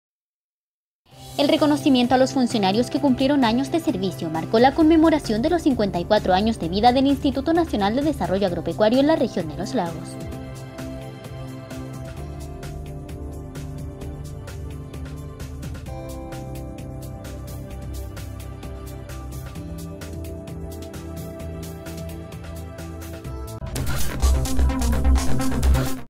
Indap Los Lagos celebró aniversario institucional con reconocimiento a funcionarios
Indap cumplió 54 años. Se homenajeó a 17 trabajadores.